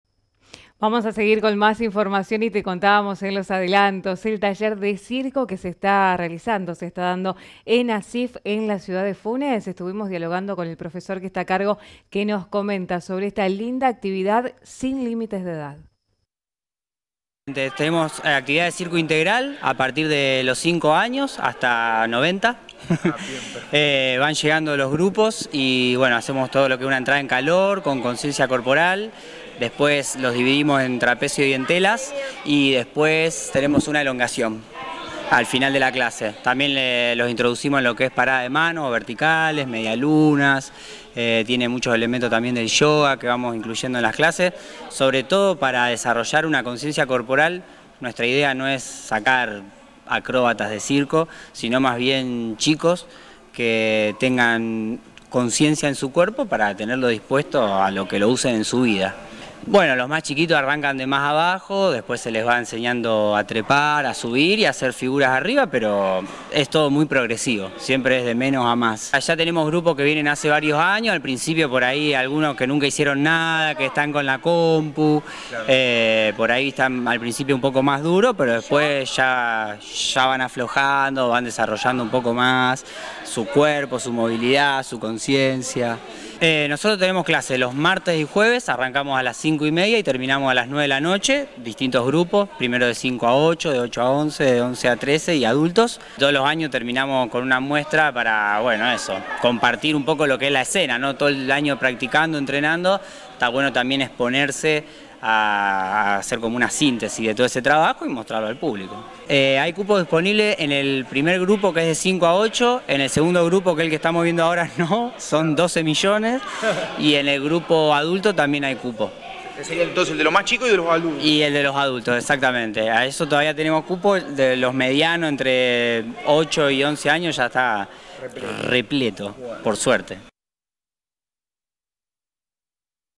Taller de Circo en ACIF Funes
En las instalaciones de ACIF Funes se lleva a cabo un taller de circo integral encabezado por el profesor Manuel Valentini.